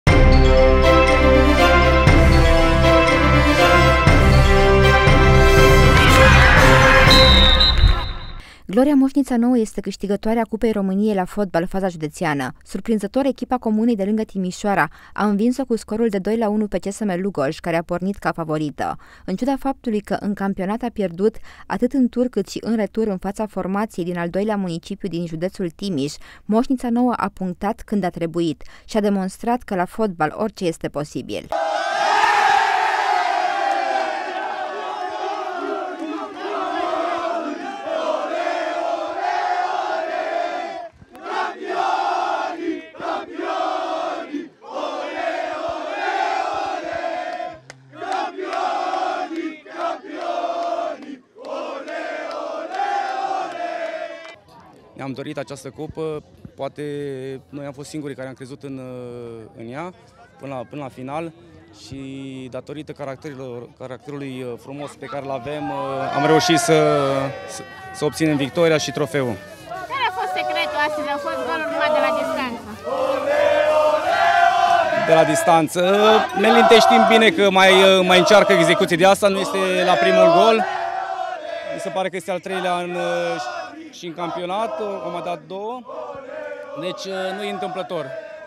Moșnița Nouă câștigă Cupa României, faza județeană
Gloria Moșnița Nouă este câștigătoarea Cupei României la fotbal, faza județeană. Surprinzător, echipa comunei de lângă Timișoara a învins-o cu scorul de 2-1 pe CSM Lugoj, care a pornit ca favorită. În ciuda faptului că în campionat a pierdut atât în tur cât și în retur în fața formației din al doilea municipiu din județul Timiș, Moșnița Nouă a punctat când a trebuit și a demonstrat că la fotbal orice este posibil.